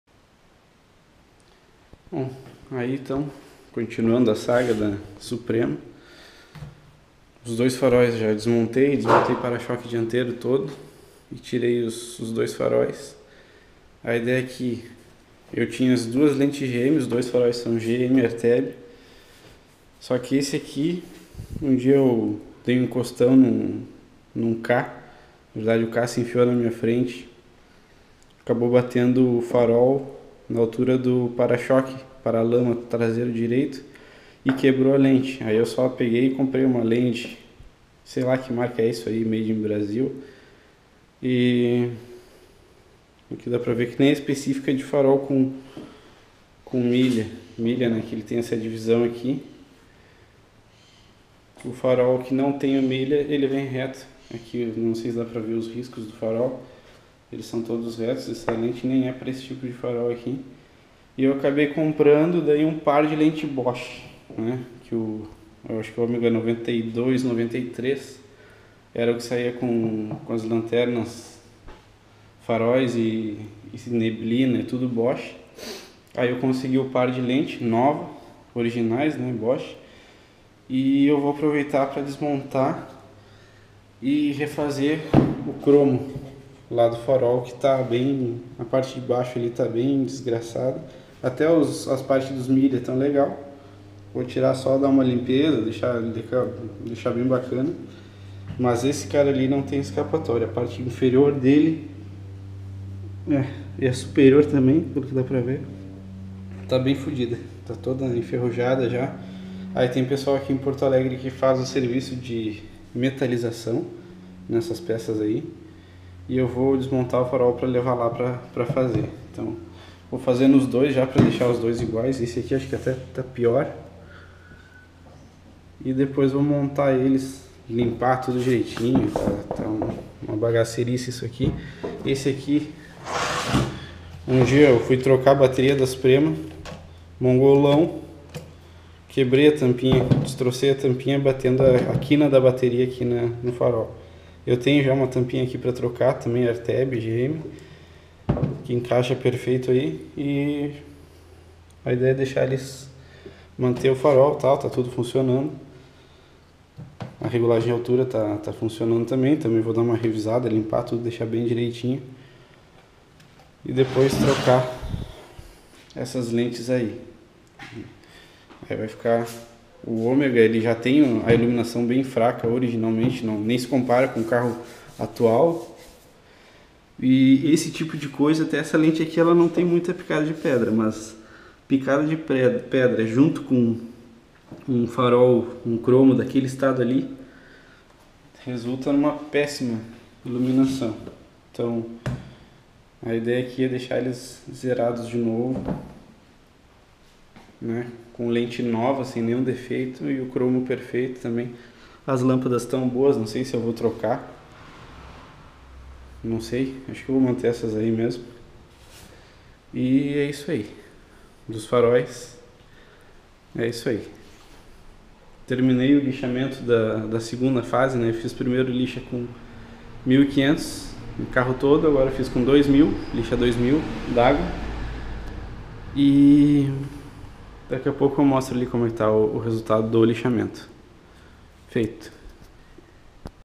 Restauração​ Suprema - #06 Recuperação dos Faróis
Breve explicação do serviço que vou precisar fazer nos faróis.